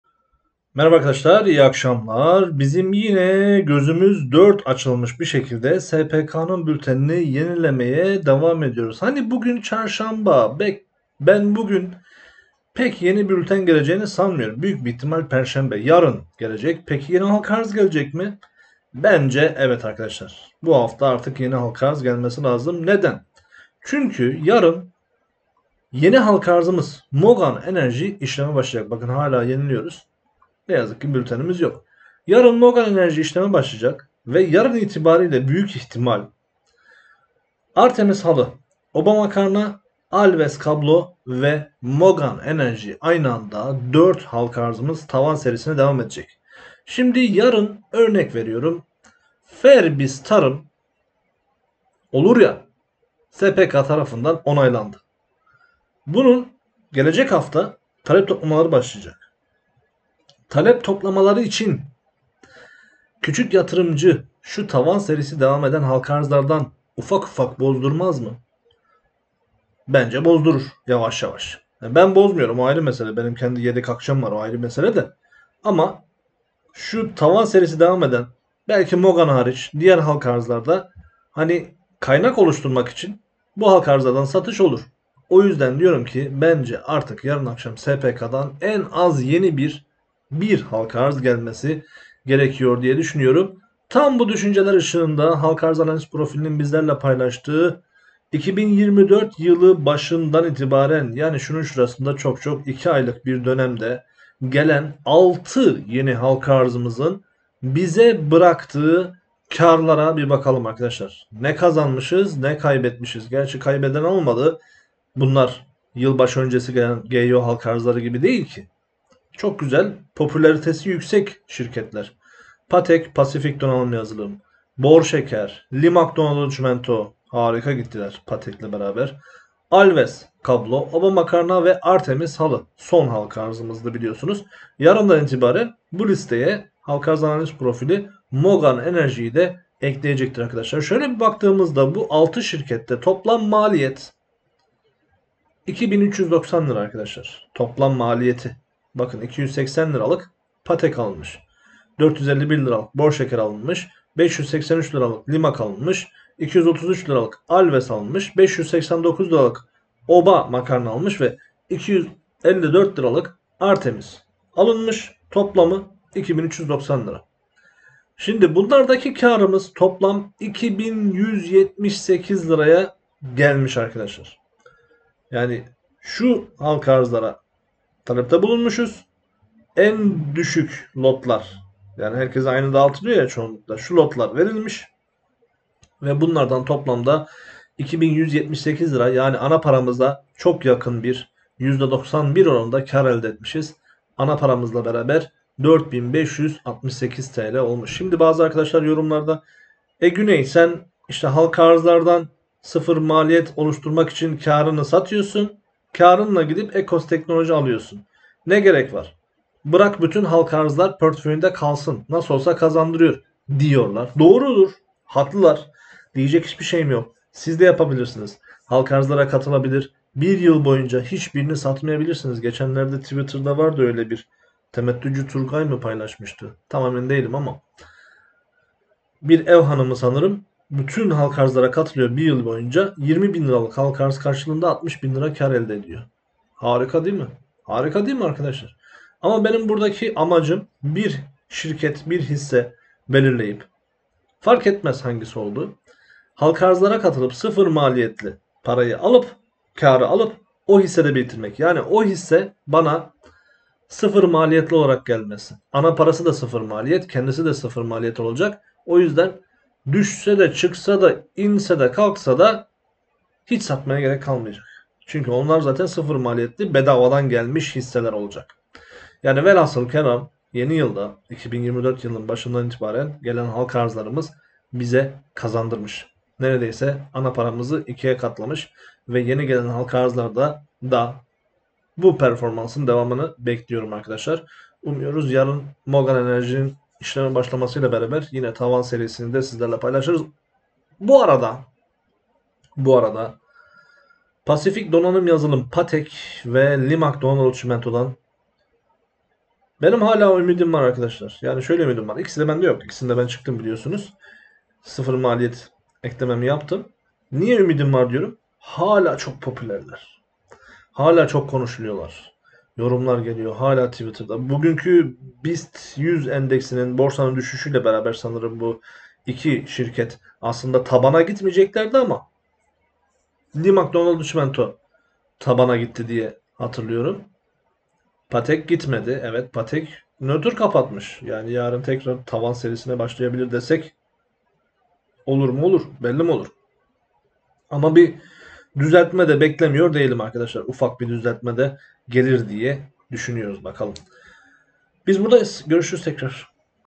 Halka Arz Hisselerinde Ana Parayı Katlıyoruz patek lmkdc obams artms alves Kazanç Oranı %91
Halka arz hisselerinin yılbaşından bu yana kazanç oranı hemen hemen ana parayı ikiye katlamış durumda. Bakalım yeni şirketlerimiz de aynı performansı gösterebilecek mi?